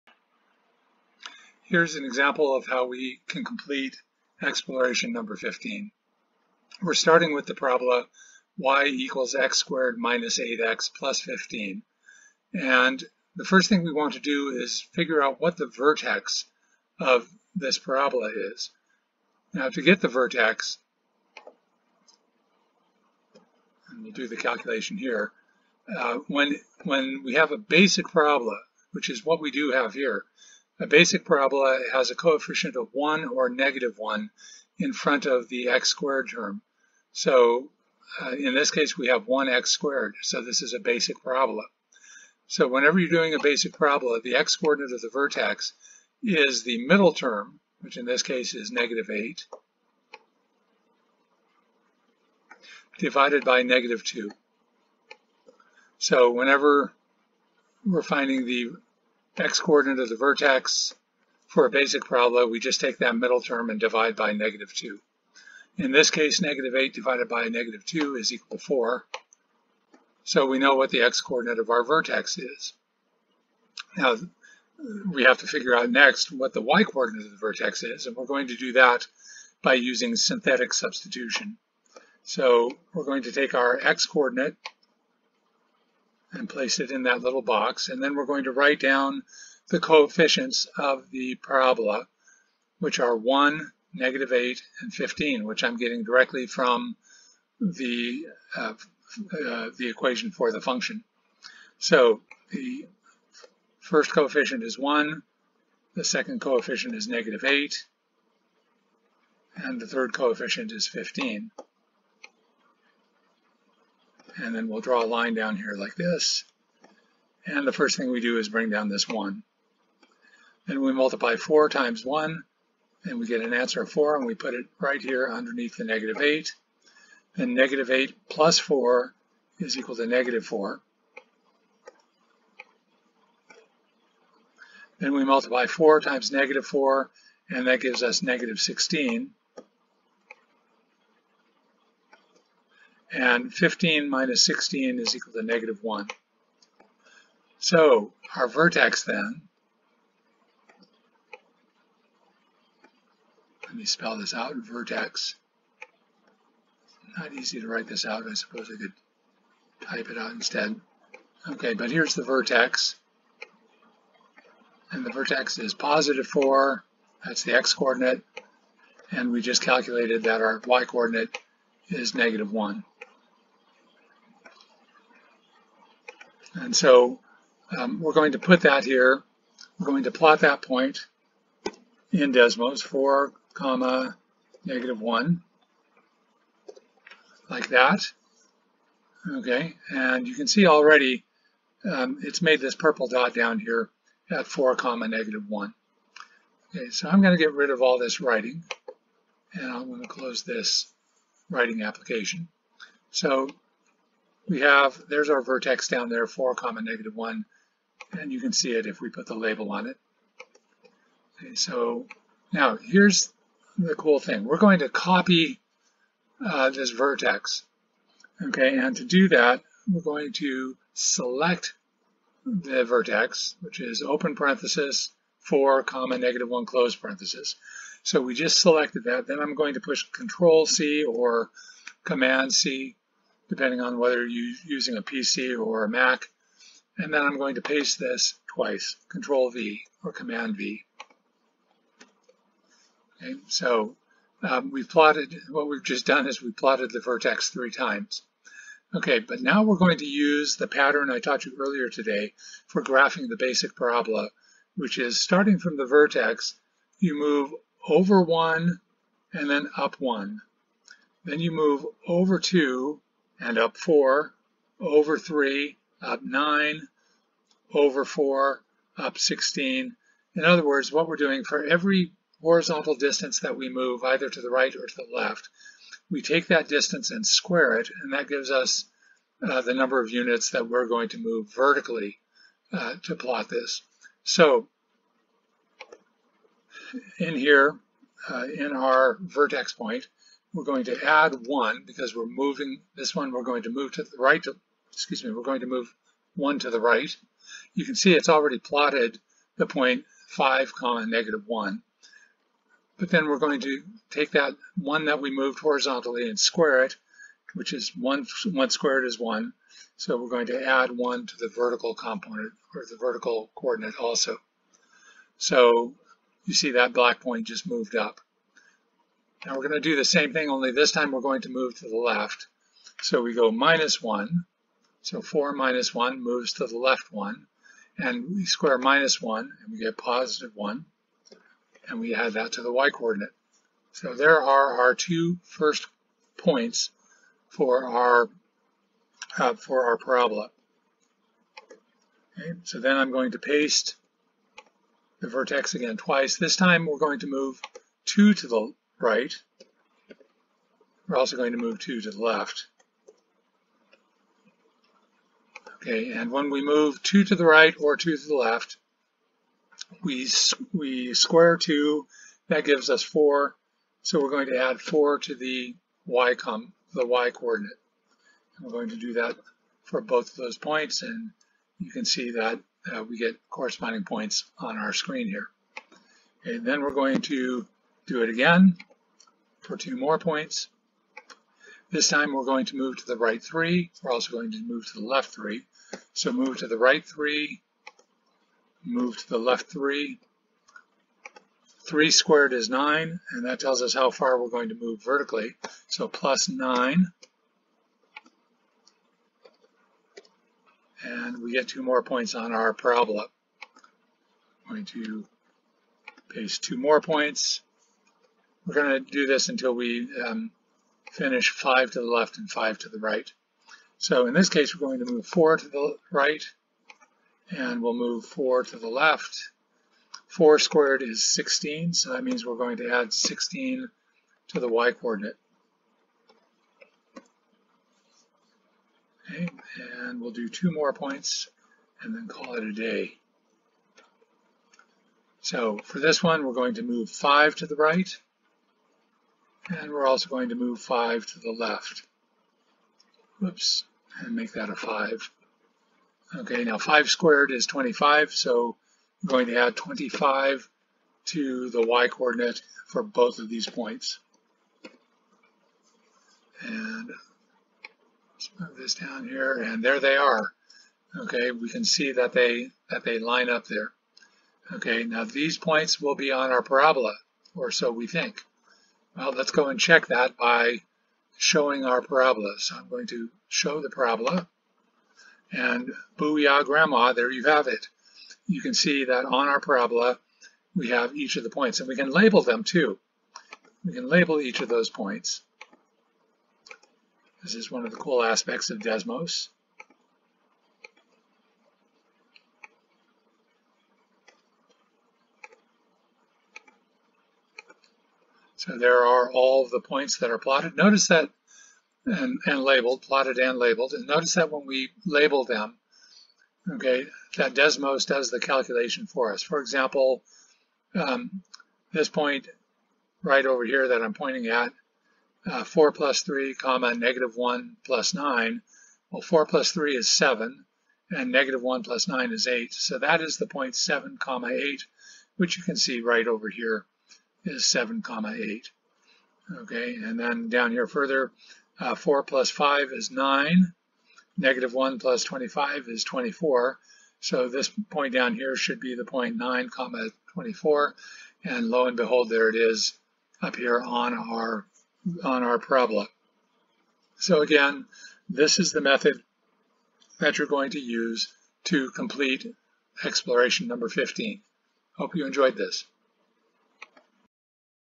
Plotting a Basic Parabola and Sample Points in Desmos
In this video we use the patterns available in the basic parabola (y = x-squared) to plot integer points on a parabola with a vertex of (4, -1).